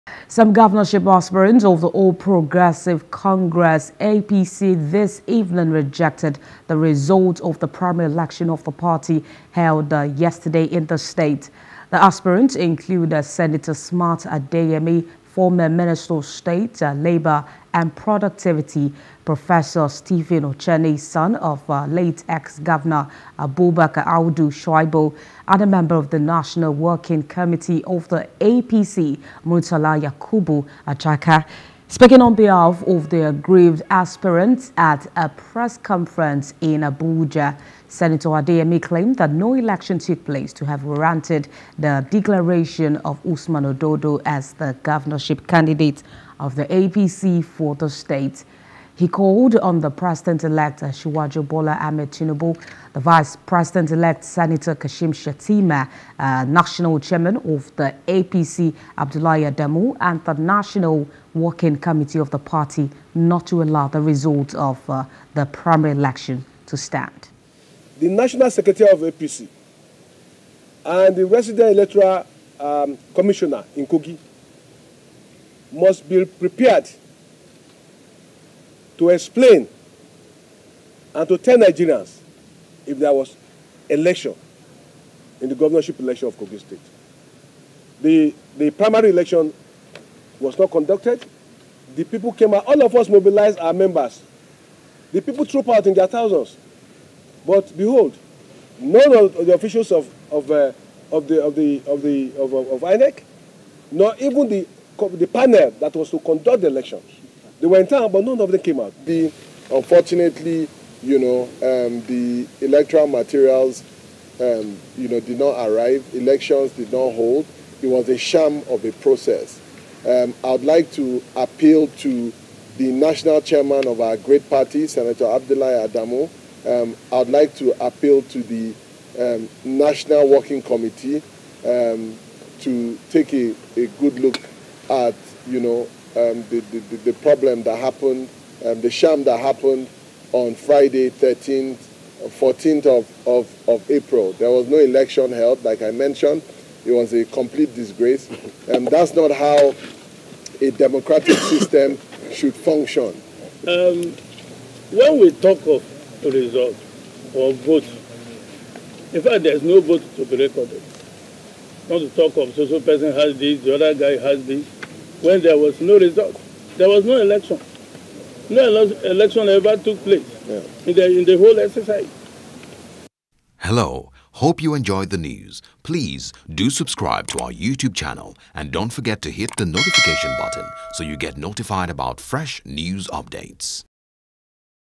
APC Primary Election: Adeyemi, Ocheni, Audu, Ajaka Reject Kogi APC Results
Some Governorship aspirants of the All Progressives Congress (APC) on Saturday evening rejected the results of the Primary Election of the party held on Friday in the state.